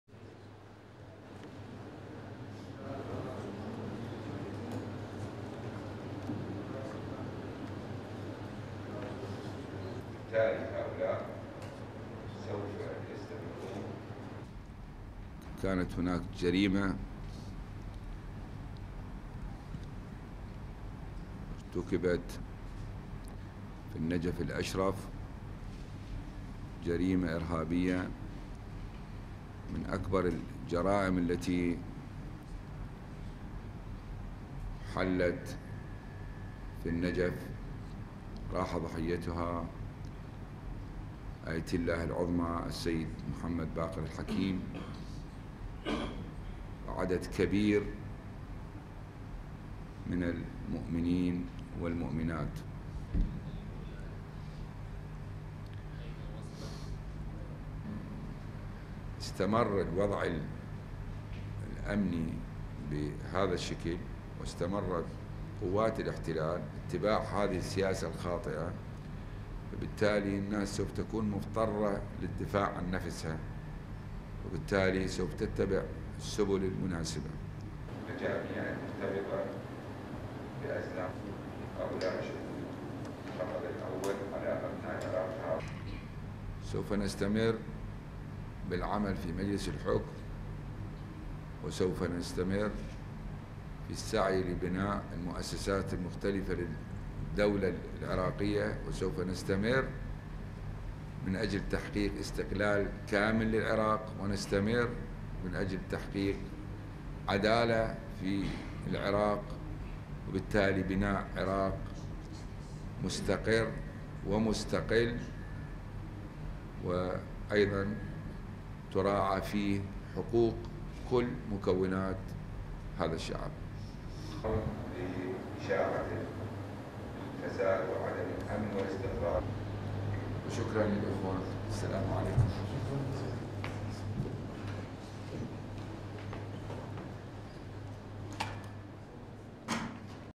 Assassinated cleric's brother on security situation
1. Abdel-Aziz Hakim walks in
2. Cutaway
3. SOUNDBITE: (Arabic) Abdel-Aziz Al-Hakim, Leader of SCIRI, Brother of Ayatollah Hakim:
"There was a crime commited in Najaf, a terrible crime, one of the biggest crimes, the killing of Mohammed Saeed al-Hakim and many other worshippers."
4. Wide of podium
5. SOUNDBITE: (Arabic) Abdel-Aziz Al-Hakim, Leader of SCIRI, Brother of Ayatollah Hakim:
"If the security situation continues like this and the occupying forces follow their incorrect policy then the people of Iraq will have to defend themselves and that will be the right thing to do."
6. Cutaway
7. SOUNDBITE: (Arabic) Abdel-Aziz Al-Hakim, Leader of SCIRI, Brother of Ayatollah Hakim:
"We will continue to work on the Council and we will continue to help build institutions in the state of Iraq. We will work for full independence, we will continue to work for justice and build a stable and independent Iraq where the people have full human rights."
8. Cutaway
9. Hakim gets up and leaves

STORYLINE:

The increasingly powerful brother of slain Ayatollah Mohammed Baqir al-Hakim said on Wednesday that Iraqis would be forced to defend themselves unless the security situation in the country improves.

The comment was an apparent reference to his Badr Brigade - the armed wing of Supreme Council for the Islamic Revolution in Iraq (SCIRI) which he established during exile in Iran.

Asked if the Badr Brigade was back in service, Abel-Aziz al-Hakim said it was more organised and would "defend Iraqi interests and will share in the rebuilding of Iraq."

Al-Hakim, a member of the US-picked Governing Council and his brother's successor as chief of the Supreme Council for the Islamic Revolution in Iraq (SCIRI), also told reporters it still was not known who was behind the huge car bombing in Najaf that killed the ayatollah and between 85 and 125 others at the close of Friday prayers.

Speaking in Najaf, Al-Hakim blamed the attack "on enemies of Iraq", saying they were followers of ousted dictator Saddam Hussein who were trying to destabilise the country.

Police in Najaf, the holiest Shiite city in Iraq, have said a number of Iraqis and foreigners with admitted ties to al-Qaida have been arrested in connection with the Friday blast.

Al-Hakim also pledged to continue working towards an independent Iraq with the Governing Council, which swore in 17 members of the newly appointed 25-member Cabinet in Baghdad on Wednesday.

The Cabinet will begin taking over many of the day-to-day duties of governing the country from the US-led Coalition Provisional Authority.